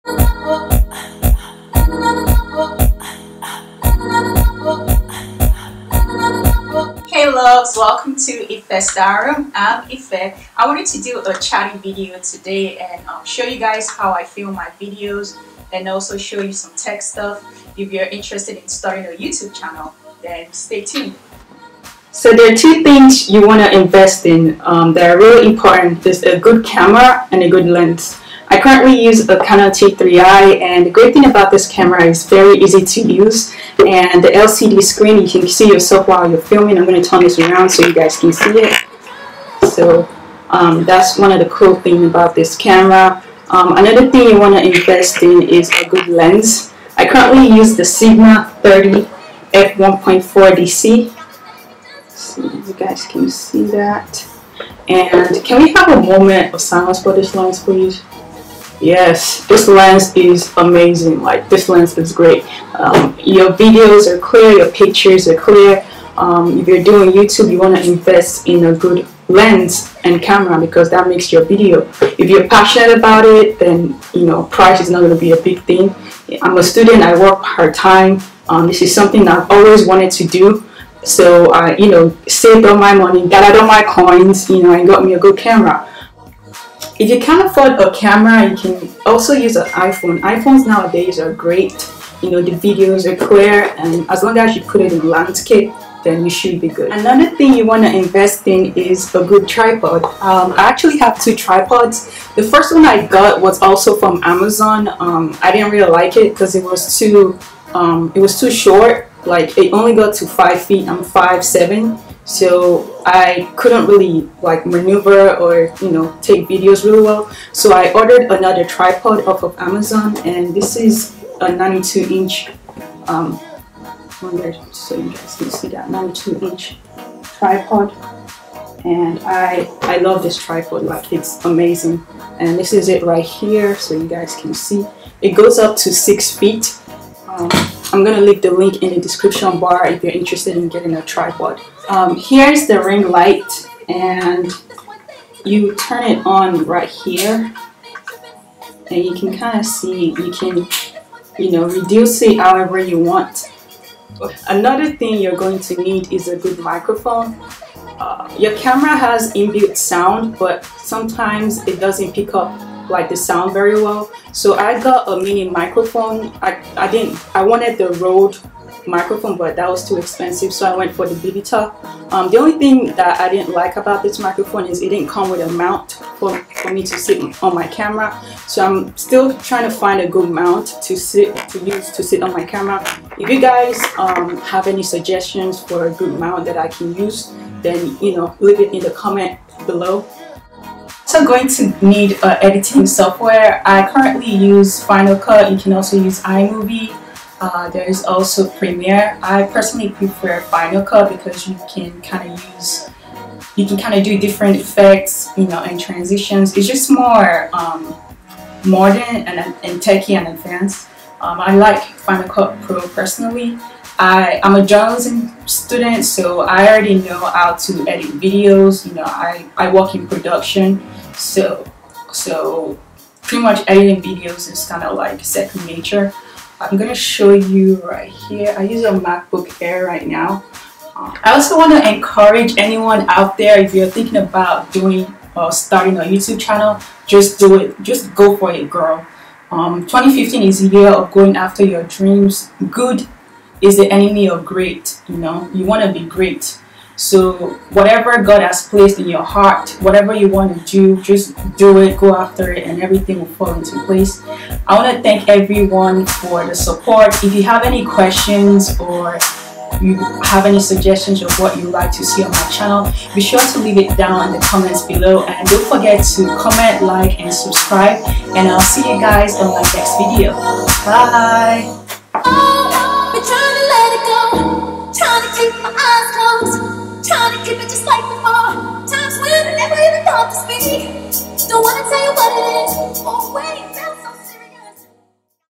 TECH HAUL : How To Start A Youtube Channel On A Budget. Camera, Lens, Tripod, Ring Light etc..
Hey Loves! Showing You Something a bit different,  How I Film My Videos and Tech Tips On Starting YouTube ChanneL. One for the gadget geeks out there!

PRODUCTS MENTIONED:
CAMERA- CANON EOS T3i DIGITAL SLR BODY ONLY

LENS - SIGMA 30MM F1.4 DC HSM ART LENS FOR CANON

RINGLIGHT- AW PROFESSIONAL 14" DIMMABLE RING LIGHT 45W Fluorescent Photo Video Studio Portrait Light 5500K w/Bag

LIGHT STAND FOR RING LIGHT

TRIPOD- 72-inch TRIPOD 3-WAY PANHEAD TILT MOTION WITH BUILT IN BUBBLE LEVELING FOR CANON

MICROPHONE-  VIVITAR MICROPHONE FOR CANON EOS

S P E C S
+ Filmed with Canon T3i Lens-Sigma 30mm f/1.4DC
+ Edited with FinalCutProx